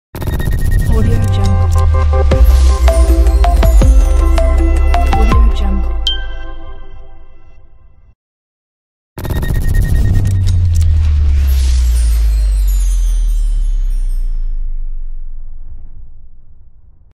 Smoke and Spikes
Easy to change color of the smoke, swtich on/off title
Sound effects included (not music)
Pre-rendered smoke
Color of the light generated from logo
Friendly to user